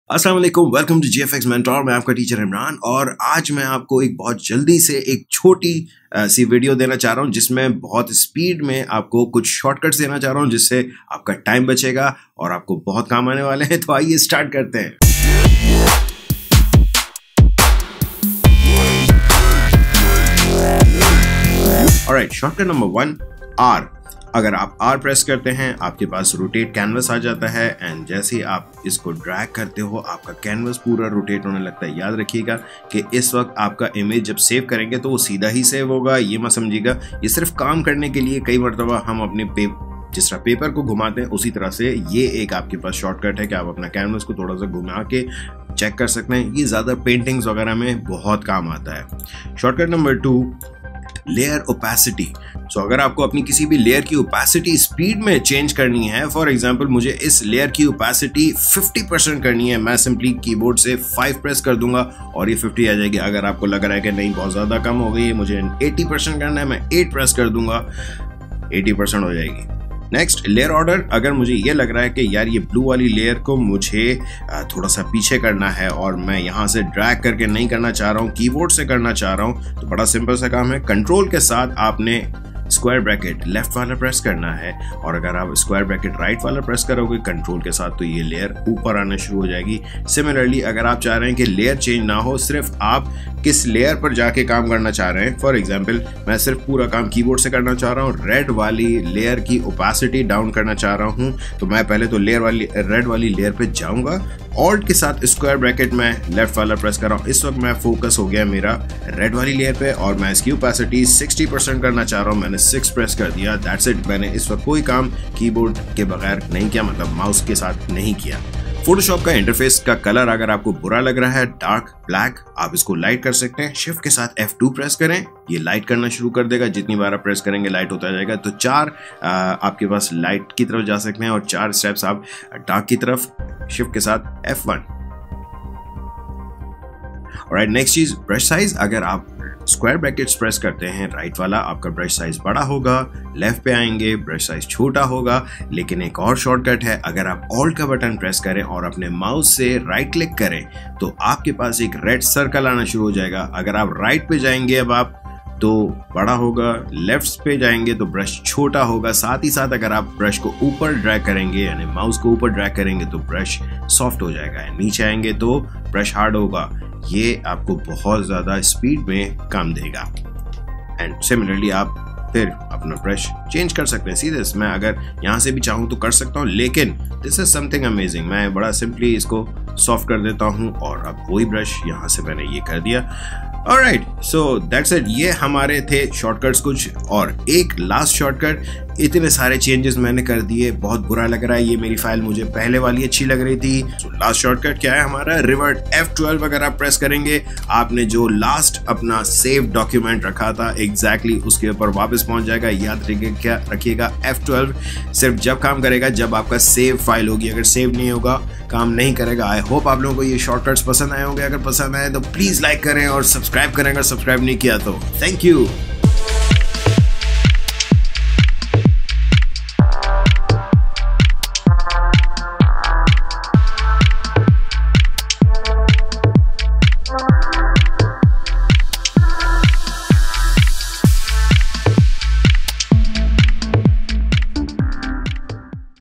6 Useful Shortcuts for Adobe Photoshop - Urdu / Hindi [Eng Sub]
Hey Everyone, Here's a quick and short video of very useful Shortcuts for Adobe Photoshop which you can use to speed up your work.

To share your design works and discuss, join Facebook group - GFXMentor Nation.

Here you will learn Adobe Illustrator Complete Course in Urdu / Hindi.